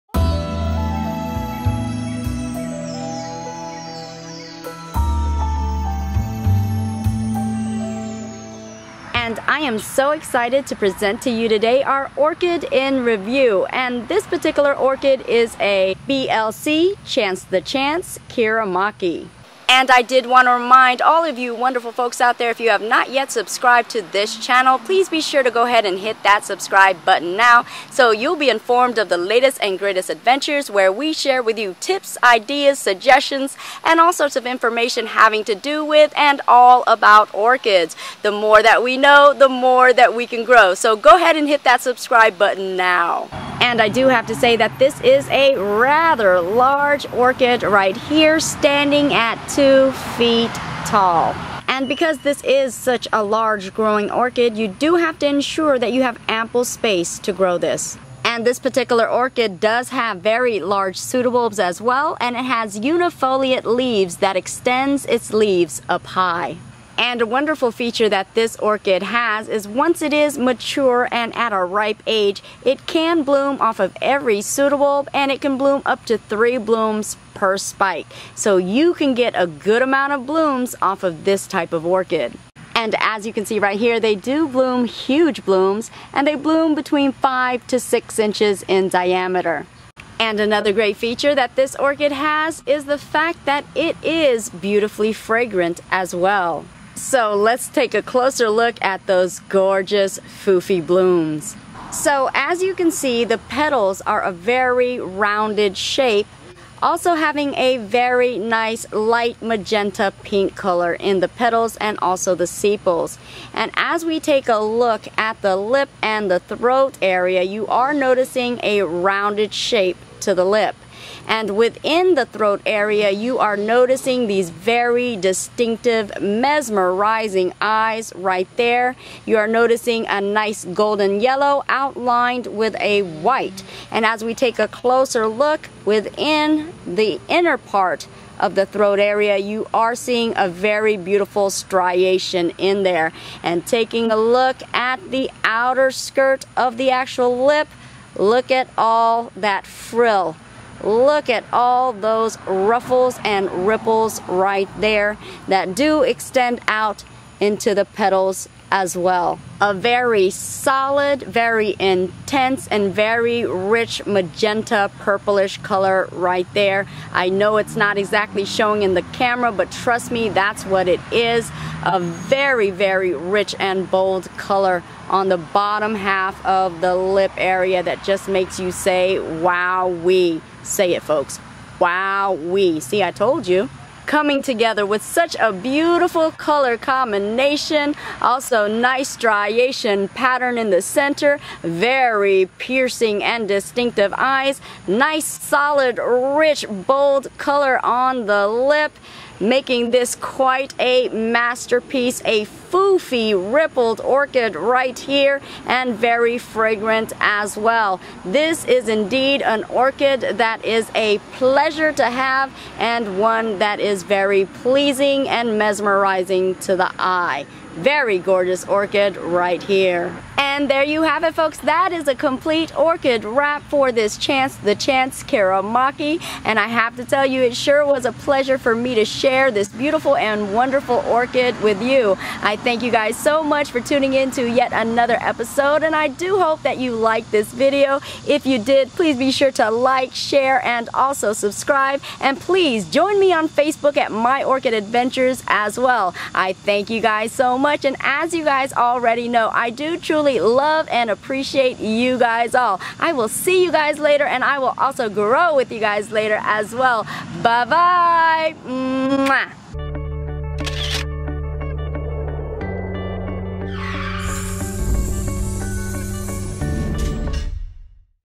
Blc. Chance the Chance 'Kiramaki' Review
Showcasing the beautiful and fragrant Blc. Chance the Chance 'Kiramaki'.